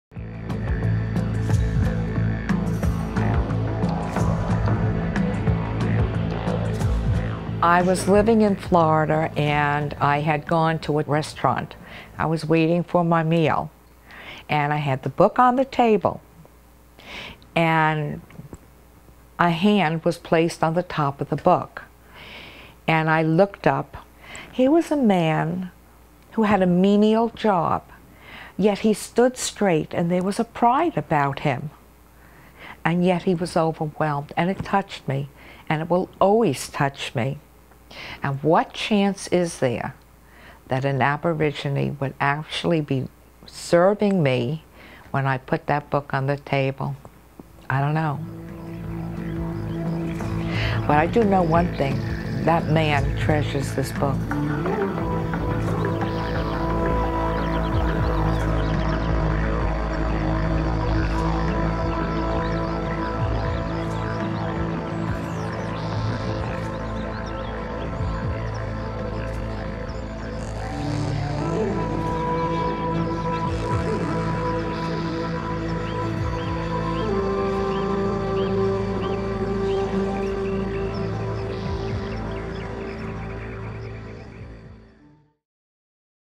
Barbara Bixon Carter and "Wisdom Man"
Attention, Oprah!
Barbara Bixon Carter (pen name, A.R. Allen) wrote The Chocolate Brown Social Club, The House of Cupcakes, and If You Really Loved Me You'd Take Out the Garbage.

Much of her work looks at the humorous side of life and she's good at it.

She bought a copy of "Wisdom Man" by Camilla Chance at a Women's Writing Guild conference in New York and that's where this story began.

What are the chances that a book on a restaurant table would move a server to tears?